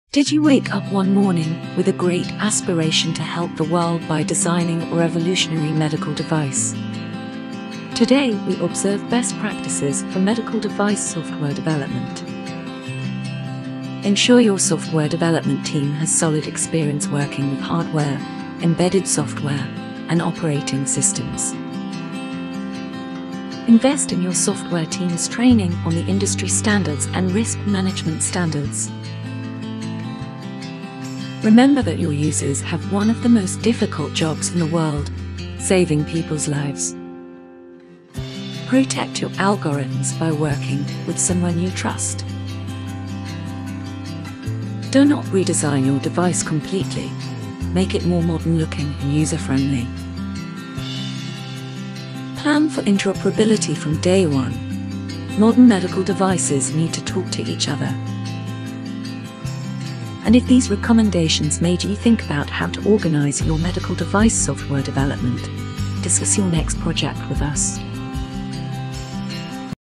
Software for Medical Devices: 6 Practices that You Should Not Overlook | Auriga
Did you wake up one morning with a great aspiration to help the world by designing a revolutionary medical device? Or are you in charge of a multibillion-dollar corporation with hundreds of medical devices on the market under your belt? Or perhaps somewhere in between?

In our short video we share some of the lessons we have learned over the years of working on medical device software projects. Here are our six best practices for software development for medical devices. We hope you will find them useful!